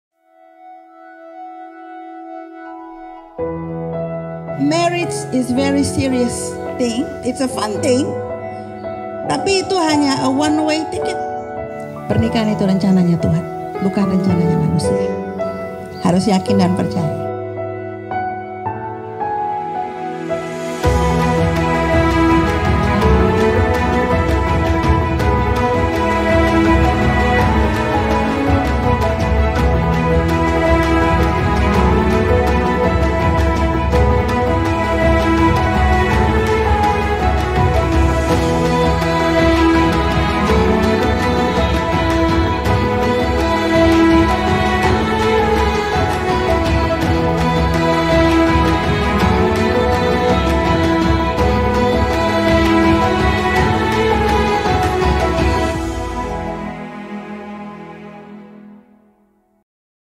Teaser Wedding Sheila & Tjhin by Alienco Photography
Video pernikahan dan foto pernikahan oleh alienco photography
specialist wedding and prewedding bogor jakarta depok bandung